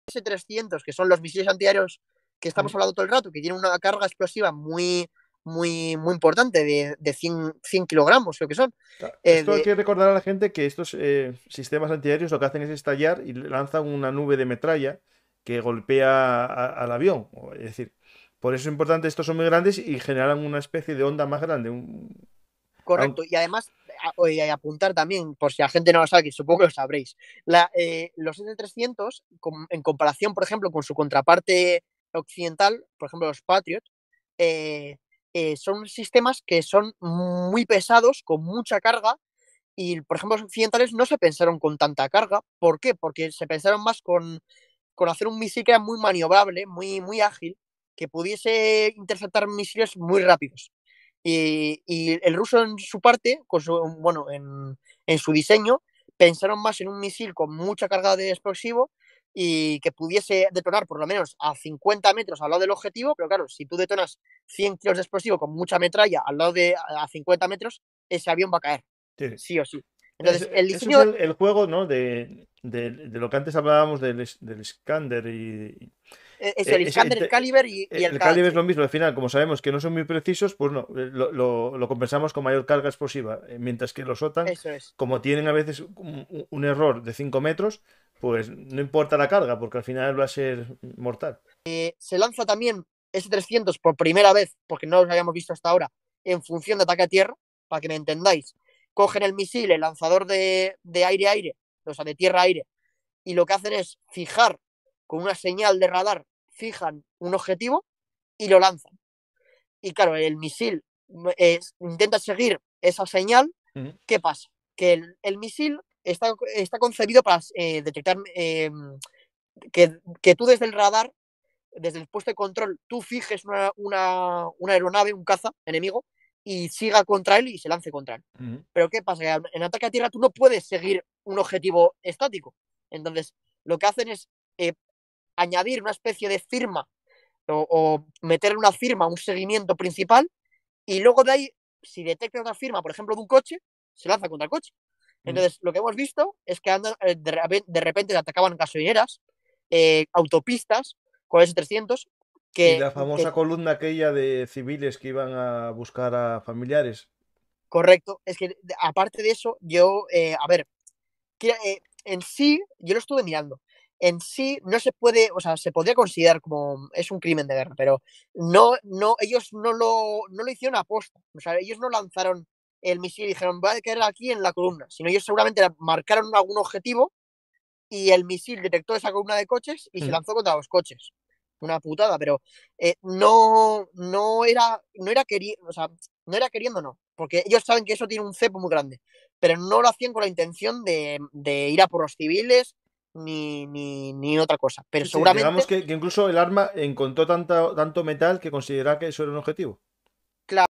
EL S-300 EN USO TIERRA A TIERRA. ¿A falta de misiles crucero un antiaéreo sirve?
Descubre cómo los rusos usan los sistemas antiaéreos S300 en configuración de ataque a tierra.
Y te invito a ver el programa completo “CAMPAÑA AÉREA RUSA EN UCRANIA”  ¿Por qué no lograron el dominio de los cielos? *HUGO ALFONSO*